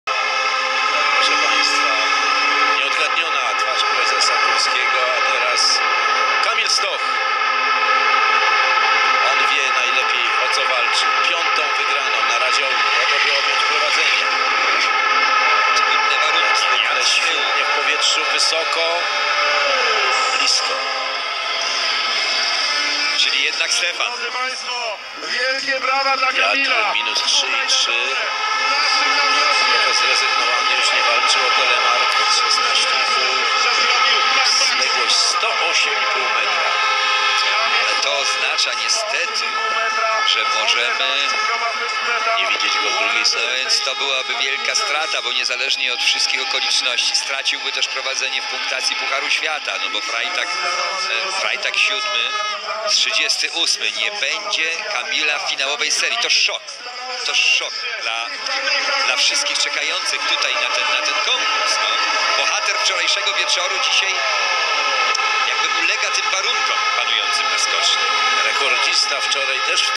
Kamil Stoch tylko 108.5 m. Nie będzie go w finałowej serii
Nieudany skok Kamila. Zabraknie go w finałowej serii konkursu indywidualnego w Zakopanym.